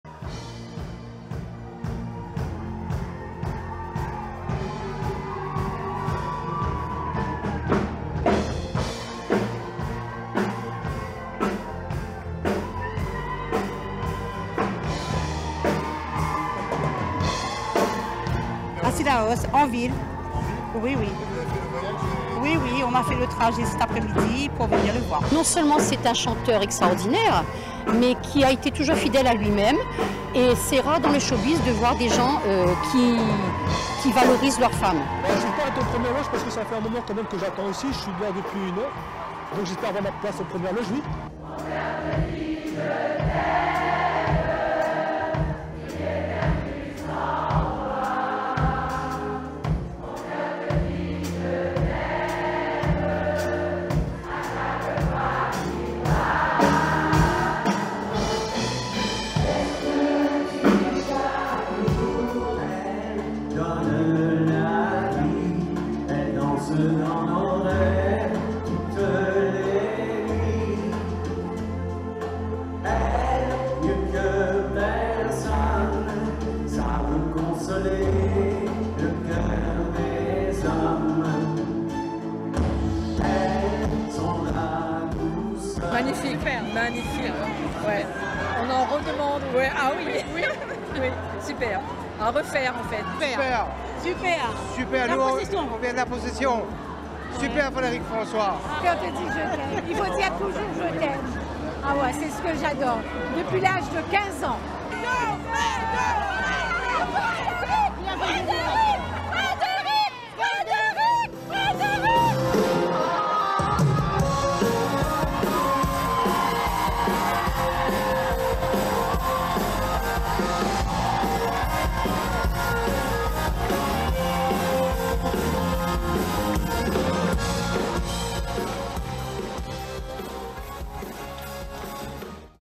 Retour en images sur les soirées Frédéric François
Fantastique, exceptionnel! les adjectifs ne manquent pas pour qualifier les deux grandes soirées de concert de Frédéric François, le wee- end dernier à Saint Joseph saintjo974.
Deux concerts à guichet fermé! le public a répondu largement présent bien en amont!
Les spectateurs pas déçus de la performance de l'artiste qui chante l'amour à l'italienne !

Vous y étiez ? partagez-nous votre sentiment en commentaires !